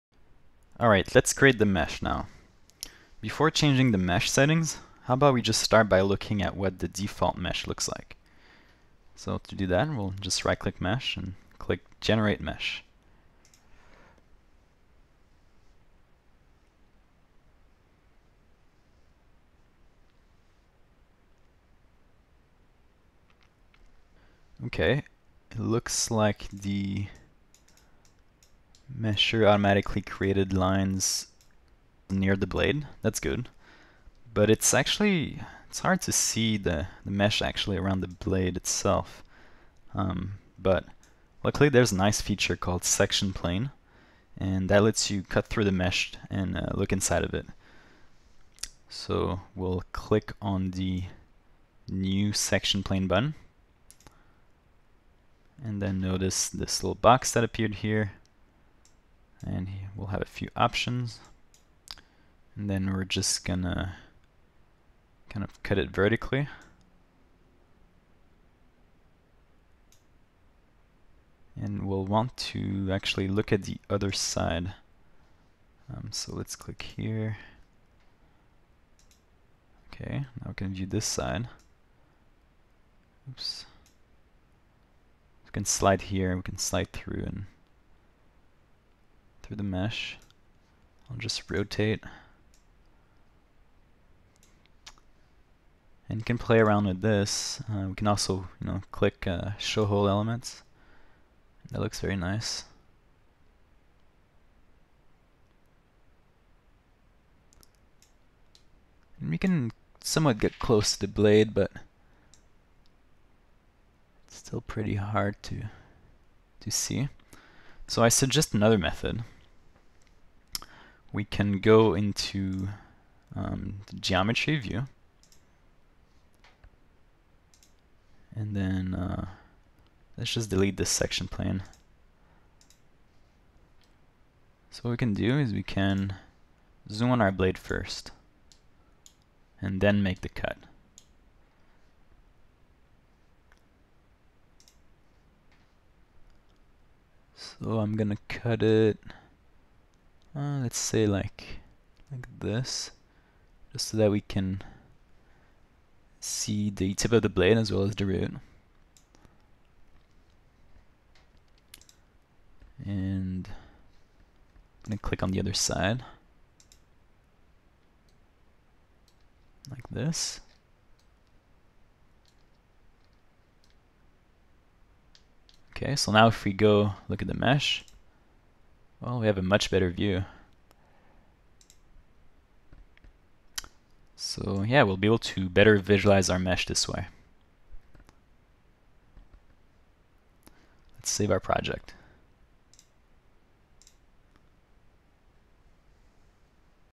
SimCafe: Wind Turbine Blade FSI (Part 1), Section Plane
This video is part of the Wind Turbine Blade FSI tutorial on SimCafe.org. Please click on the following link to access this tutorial.